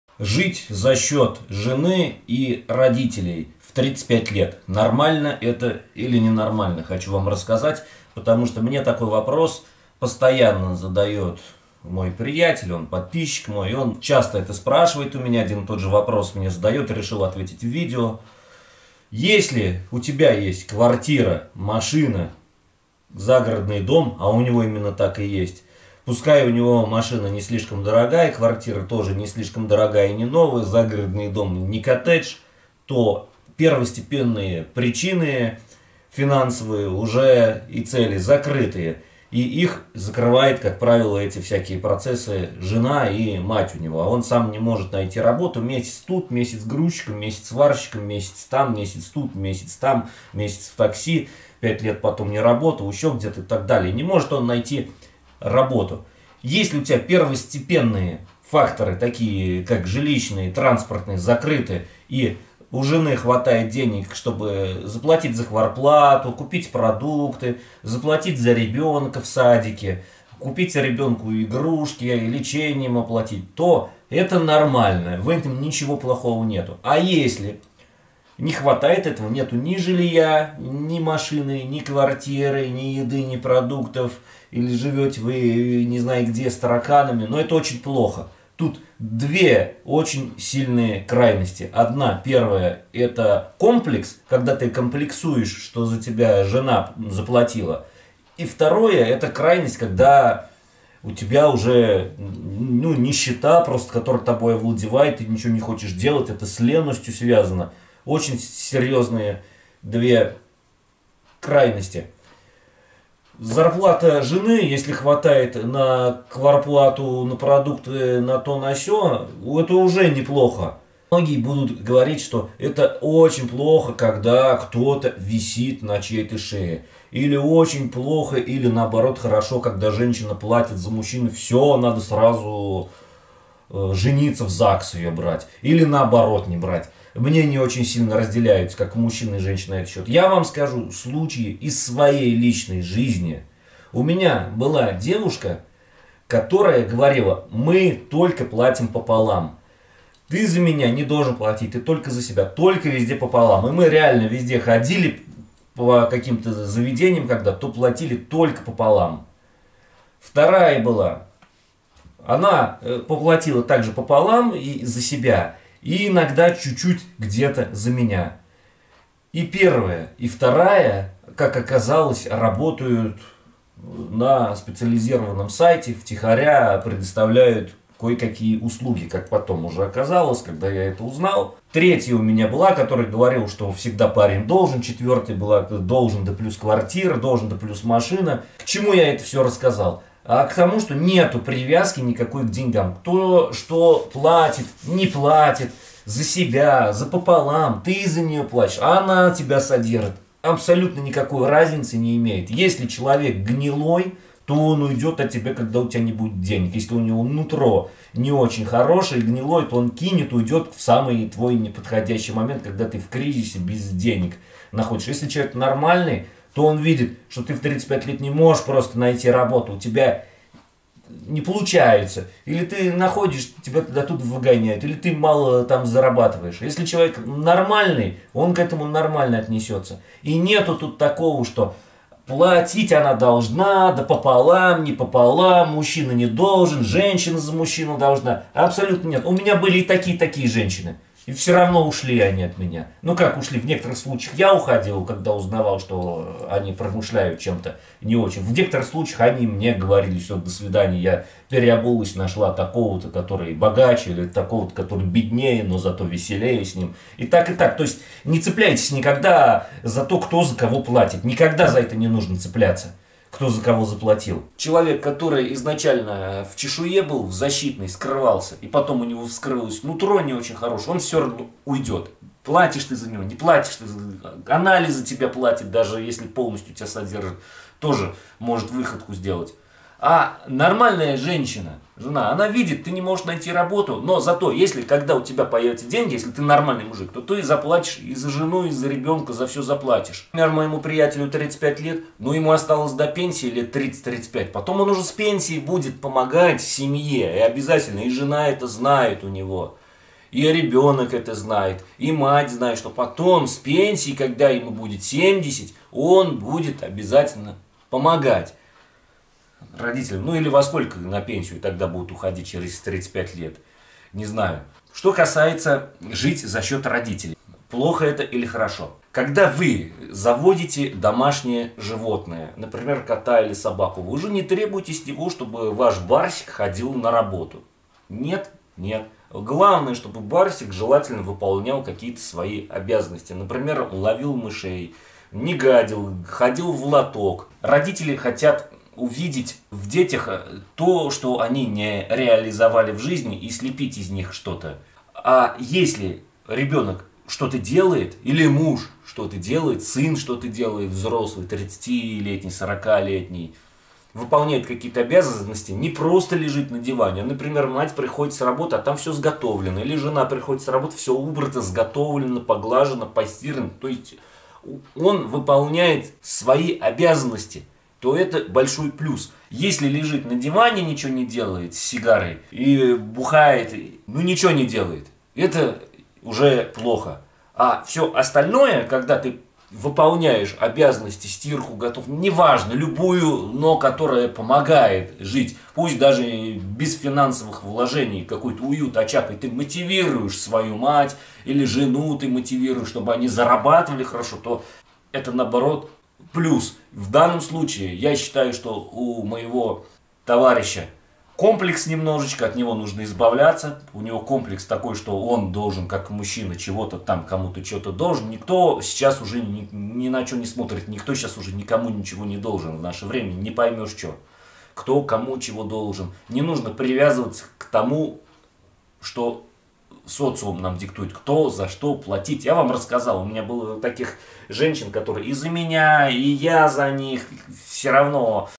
Жить за счет родителей и жены в 35 лет. Это нормально или нет?
жизнь за  счет родителей